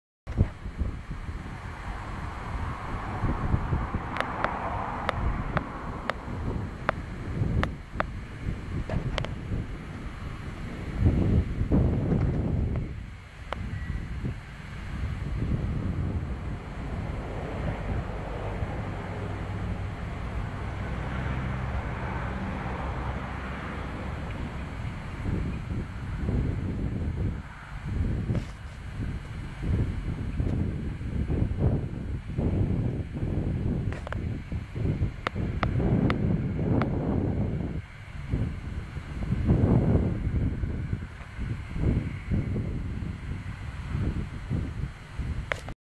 Rowing Hay in Maynooth
A quick clip I took of a Massey 165 rowing hay at Moyglare Maynooth

 Its not a great clip, as I was changing some settings on the phone